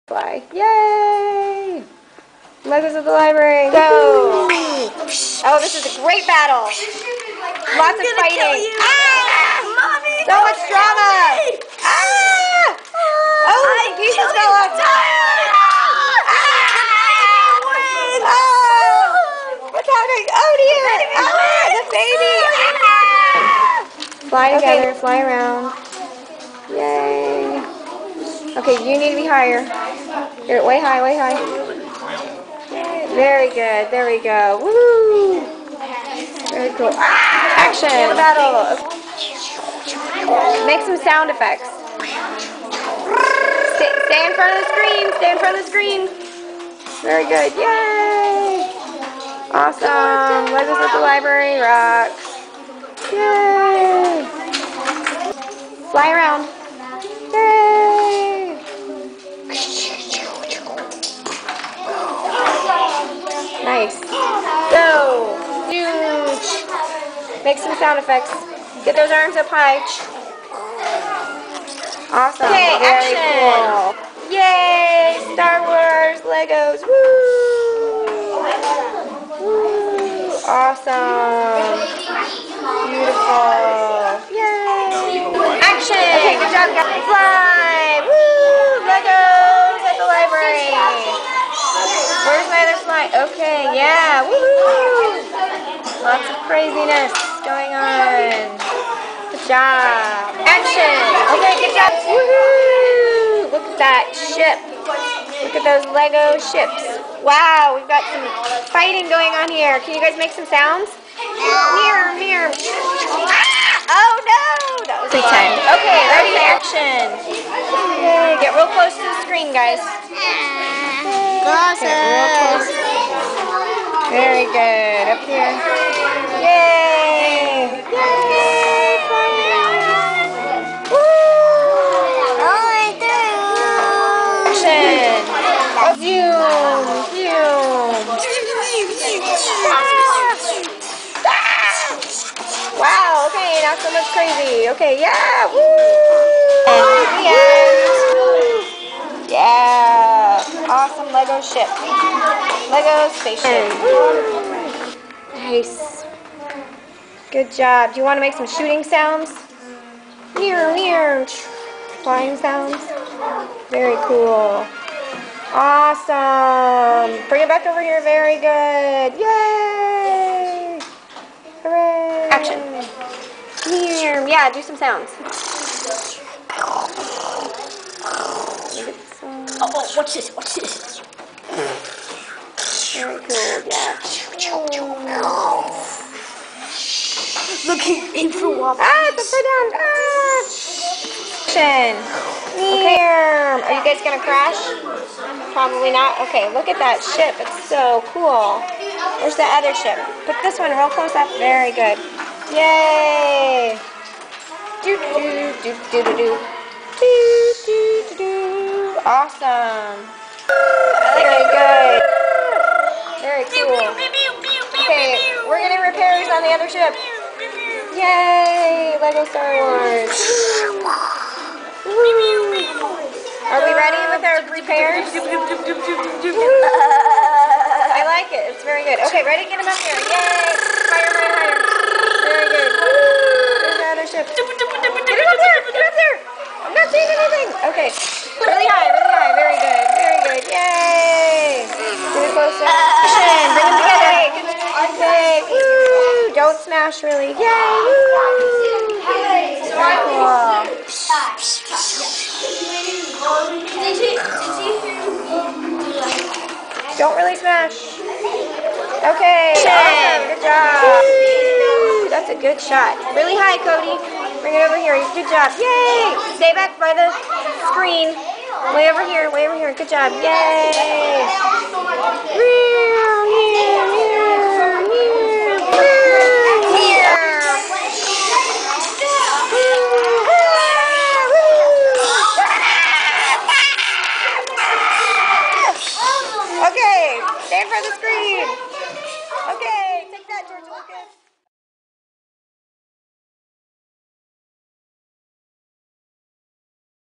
Lego Star Wars - 6/28/12
Sixty-seven children participated in the Mooresville Public Library's three Lego Star Wars programs on 6/28/12!  They had a great time looking at awesome lego creations, creating their own space ships, and making this video!  This was part of the library's Dream Big - Read, Summer Reading Program!